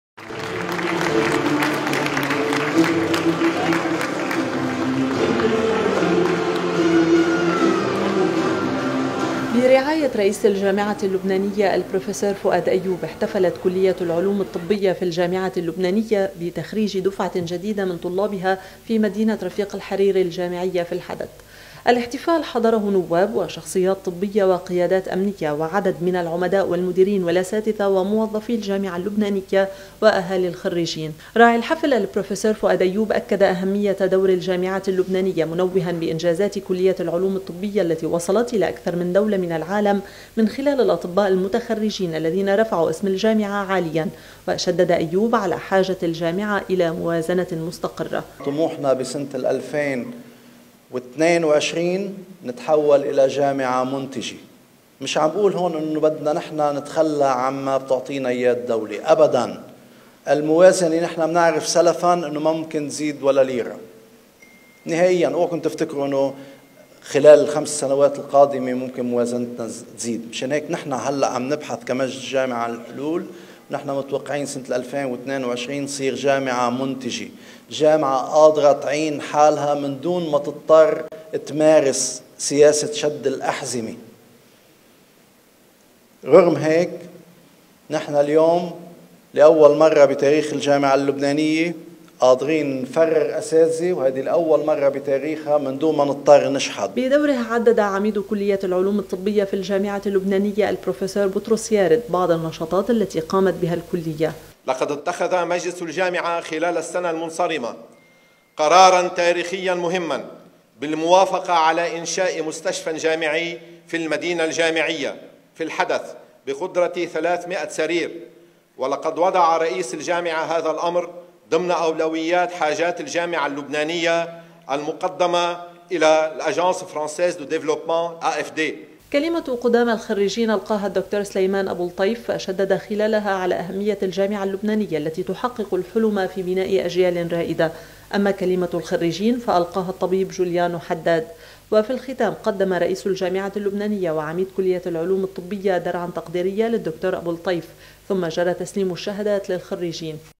حفل تسليم الشهادات لخريجي كلية العلوم الطبية في الجامعة اللبنانية
المصدر: NBN